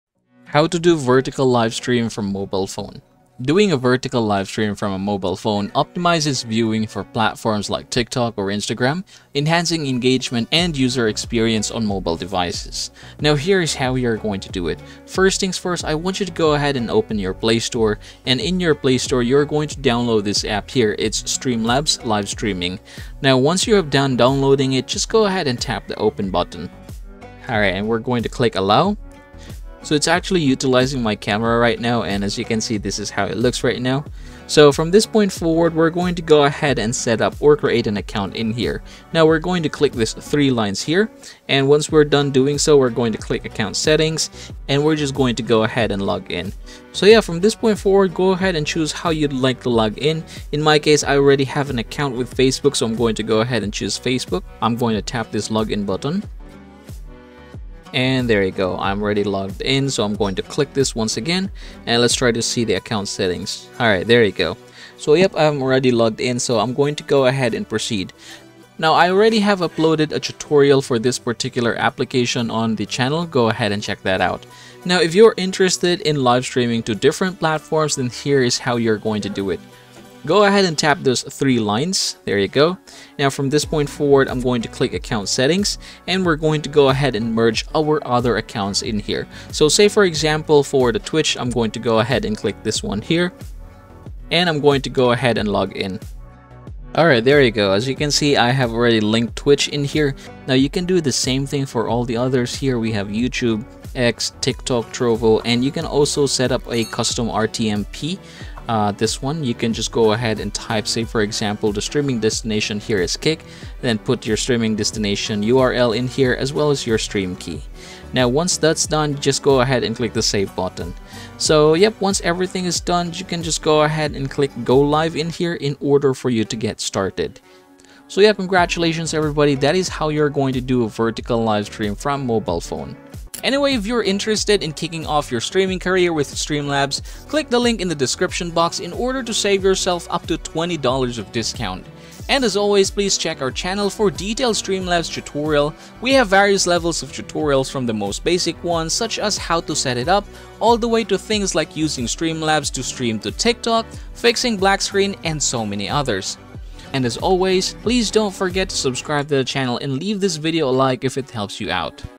How to Do Vertical Live Stream From Mobile Phone (YouTube, Twitch, Kick & More!)
Did you know how to do vertical live stream from mobile phone Youtube, Twitch, Kick? Here's a tutorial video of how to do vertical live stream from mobile phone YouTube, Twitch, Kick &more!